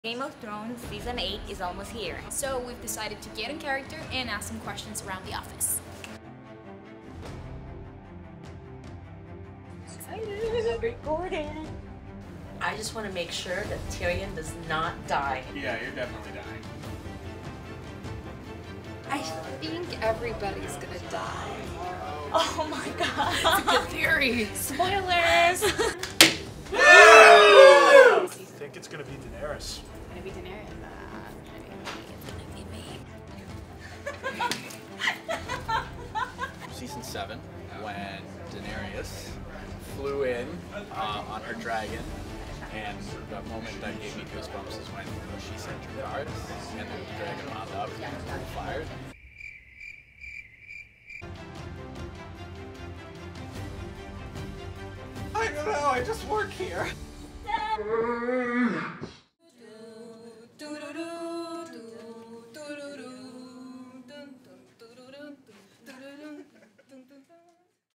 Game of Thrones: Start.ca Edition
Spoiler Alert!

What's gonna happen on the last season of Game of Thrones (available on Start TV)? We asked our team...